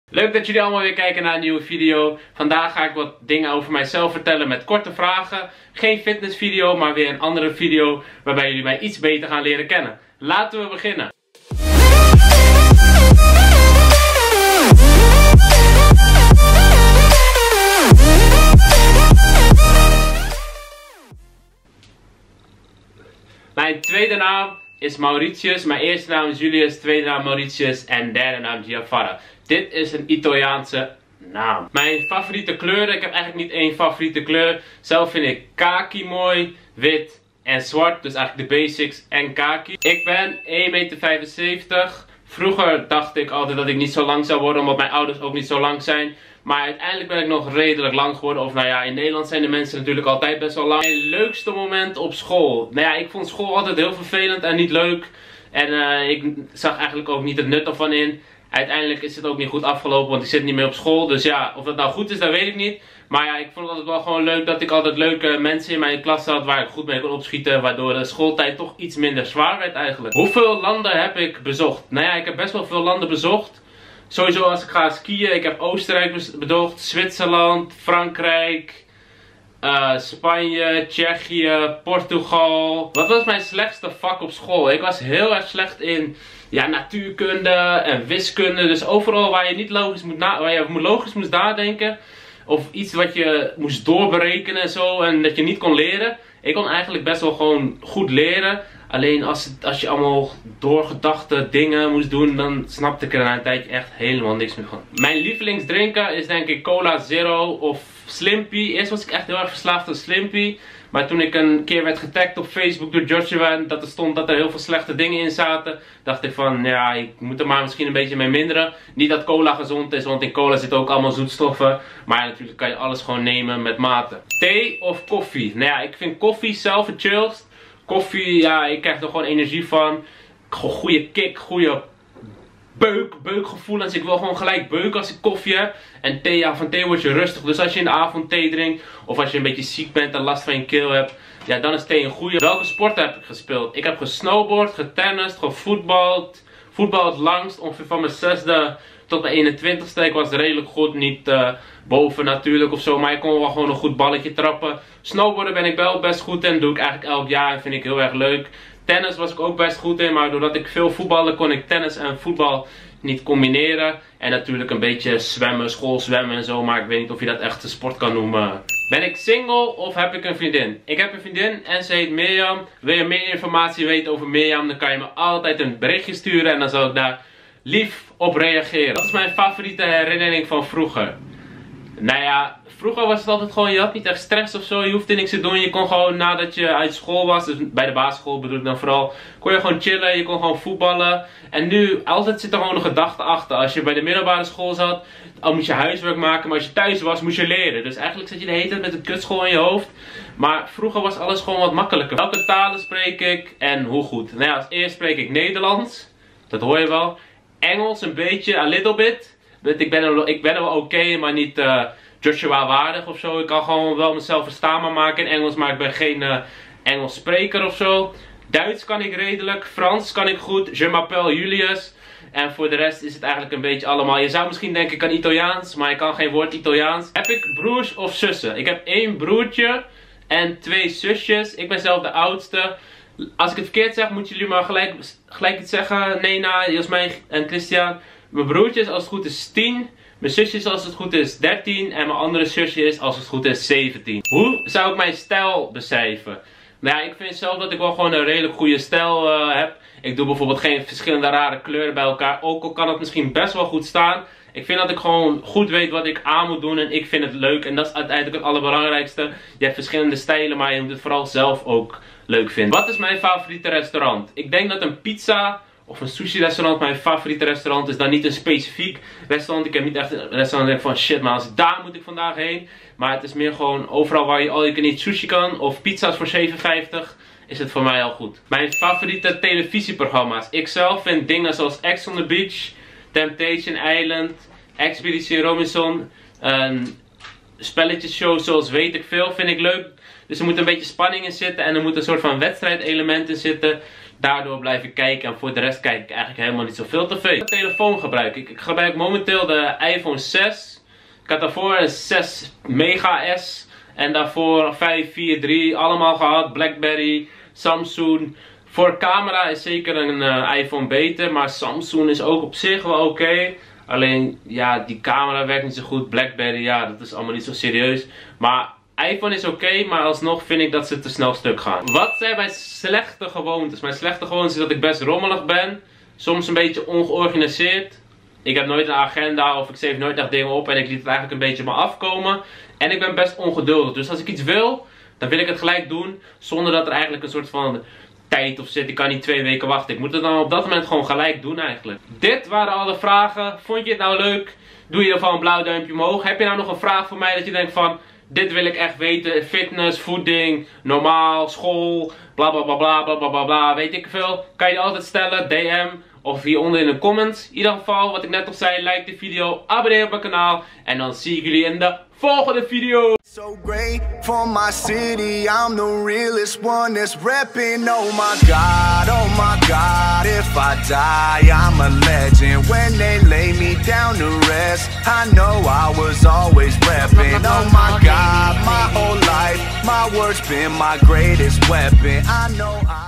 GET TO KNOW ME! - JULIUS GIAVARRA
LEER MIJ BETER KENNEN!
Leuk dat je weer kijkt naar een nieuwe video. In deze video ga ik korte vragen over mijzelf beantwoorden. Hierdoor leren jullie mij weer wat beter kennen!

Online coaching voor maar €9,99 per maand!

10% korting met 'Giavarra10'